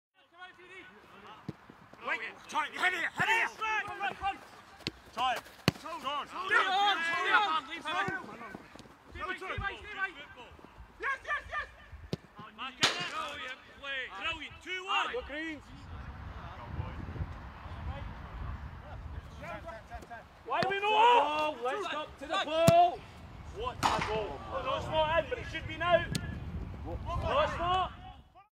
Training Tekkers: A ripper from the Skipper and Cyrus Christie goes close
Watch as Carl Baker shows the City squad how to finish from long range and Cyrus Christie goes so close.